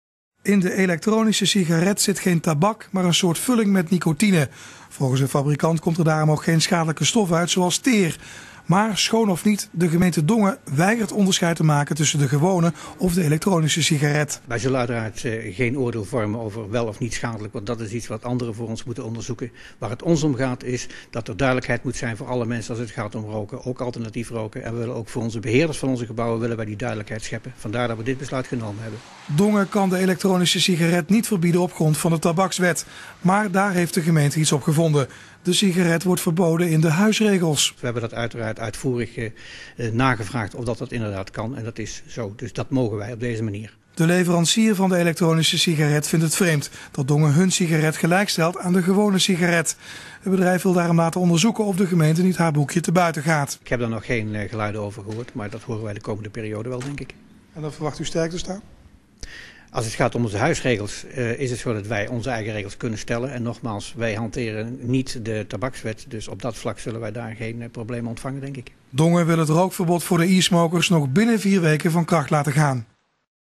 Dongen legt SuperSmoker aan banden
Dongen legt het roken van de SuperSmoker aan banden. Het is daarmee de eerste gemeente in Nederland die het roken van de elektronische sigaret verbiedt in openbare ruimtes.